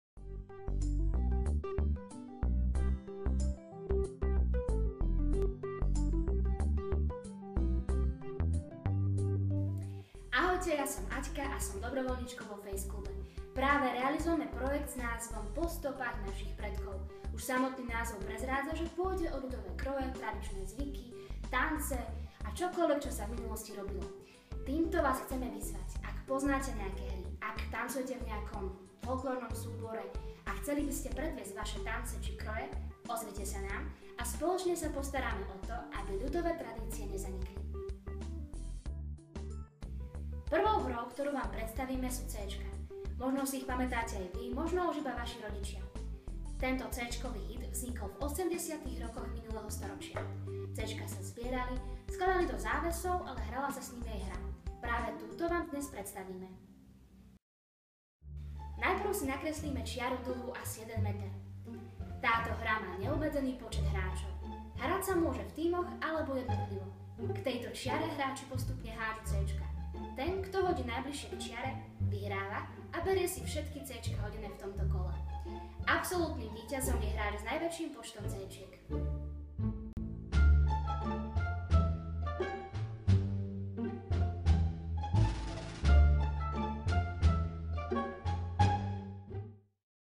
Hry našich predkov #1 | Céčka
Pokiaľ si nevedel, tak s týmito Céčkami sa hrávala jedna veľmi jednoduchá, no obľúbená hra. Pre zoznámenie sme pre Vás pripravili krátke video.

Projektom Po stopách našich predkov, ktorý je podporený Nadáciou pre deti Slovenska z fondu Hodina deťom, chceme dosiahnuť, aby aj dnešná mladá generácia poznala "klasické" hry, ktoré nás dokázali zabaviť na celé dni.